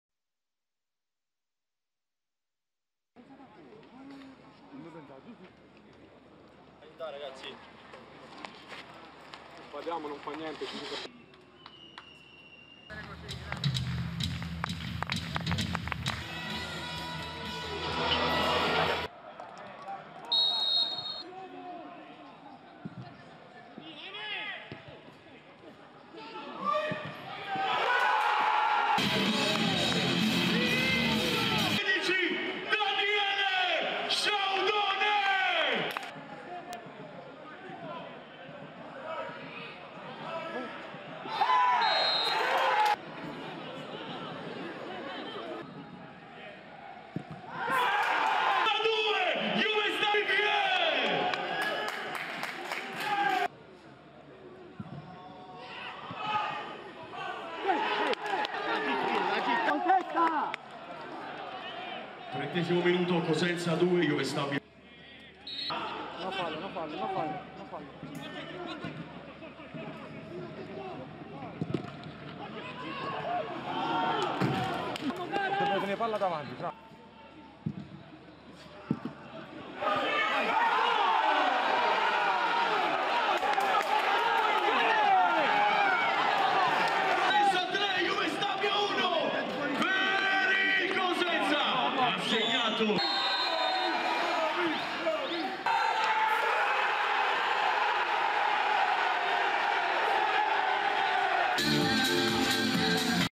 Anno 2019/20 Cosenza - Juve Stabia 3 - 1 (Highlights)
31/07/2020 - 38a Giornata di Serie B 2019/20

il tabellino

Cosenza (3-4-3): Saracco; Capela, Idda, Legittimo; Casasola, Bruccini, Sciaudone, Bittante; Carretta (dal 30’ pt Asencio), Riviere, Baez (dal 24’ st Kone). Allenatore: Roberto Occhiuzzi.

A disposizione: Perina, Corsi, Lazaar, Broh, Asencio, D’Orazio, Bahlouli, Schiavi, Machach, Monaco, Kane, Prezioso.

Juve Stabia (4-3-3): Provedel; Fazio, Tonucci (dal 27’ st Germoni), Allievi, Ricci; Mallamo, Calò, Addae (dall’11’ st Cisse); Canotto (Rossi), Forte, Di Mariano (dal 34’ st Bifulco). Allenatore: Fabio Caserta.

A disposizione: Russo, Melara, Rossi, Bifulco, Izco, Cisse, Troest, Germoni, Mastalli, Vitiello, Mezavilla, Calvano.

Reti: Sciaudone (CO) 17’ pt, Riviere (CO) (rig.) 23’ pt, Tonucci (JS) 31’ pt, Riviere (CO) 29’ st.

Ammoniti: Tonucci (JS) 9’ pt, Sciaudone (CO) 35’ pt, Di Mariano (JS) 39’ pt, Bittante (CO) 19’ st, Caló (JS) 25’ st, Fazio (JS) 34’ st.

Arbitro: Gianluca Aureliano di Bologna.